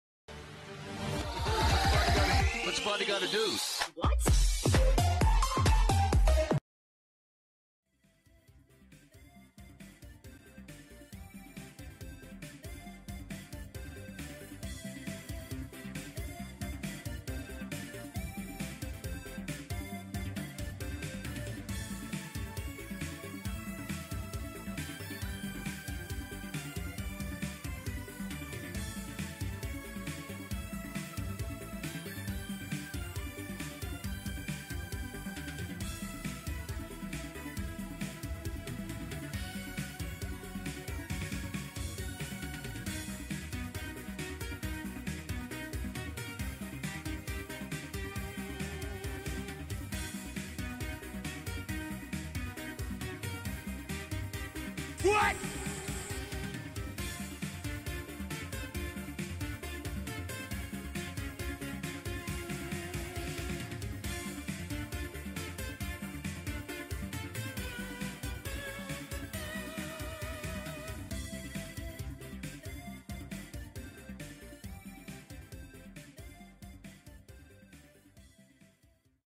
[Toreba 抓樂霸 - Huge Win #9!] Rilakkuma Chubby Fluffy XL Plushy!
[Toreba] Rilakkuma Chubby Fluffy XL Plushy!

Playing Japan's online crane game.

[Damage]
TP: 7,200 (~ SGD 11)
Number of Plays: 4

ClawMotion @ Carousell is up! If you're interested in the item, follow the link above to get the items at discounted prices!